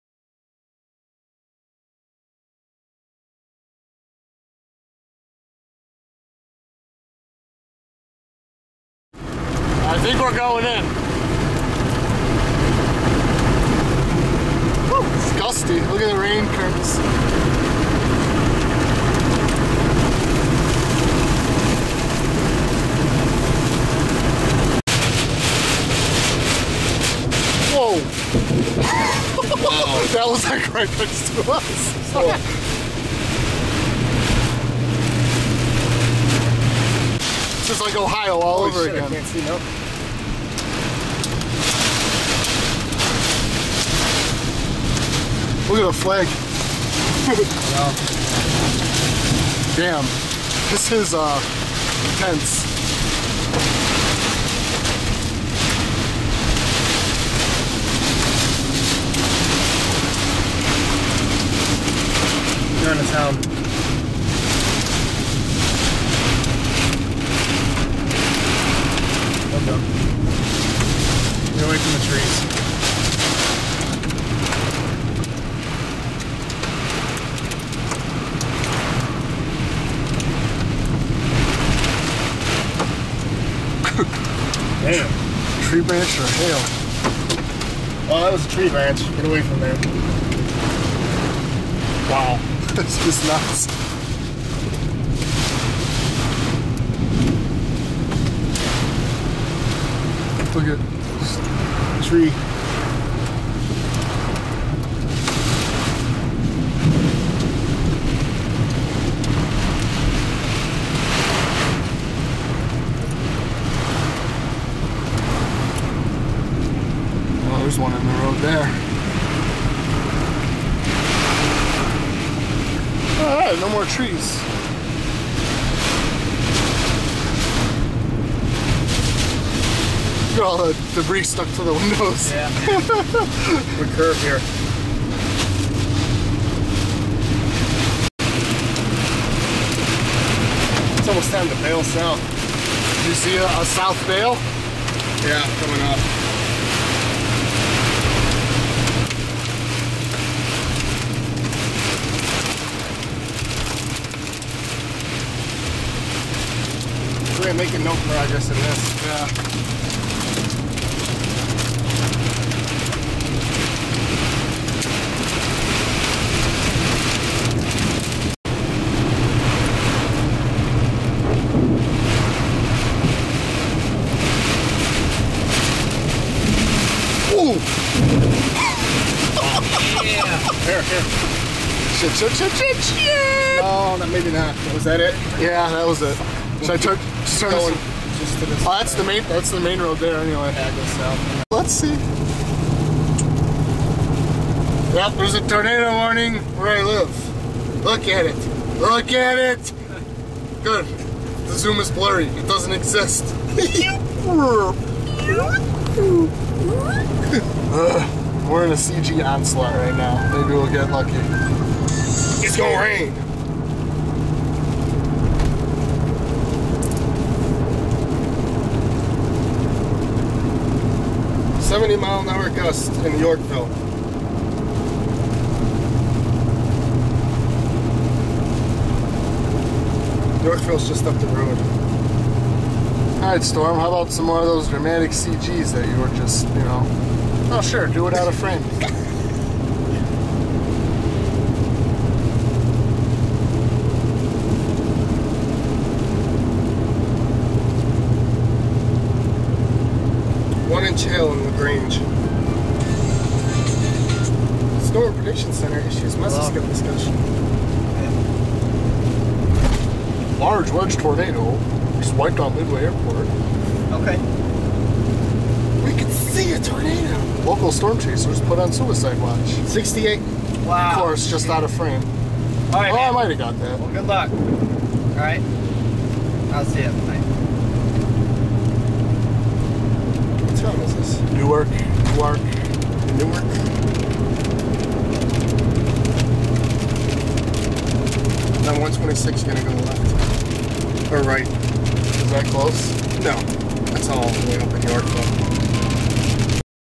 Severe Weather in Illinois 6-23-10
Trying to intercept an HP supercell near Dixon, Illinois.